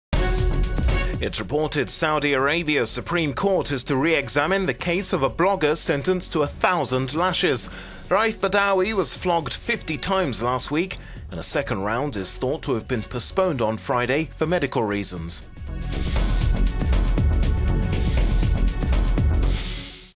Saudis to review flogging case in Supreme Court
It is reported Saudi Arabia's Supreme Court is to re-examine the case of a blogger sentenced to 1,000 lashes. Raif Badawi was flogged 50 times last week and a second round is thought to have been postponed on Friday for medical reasons.…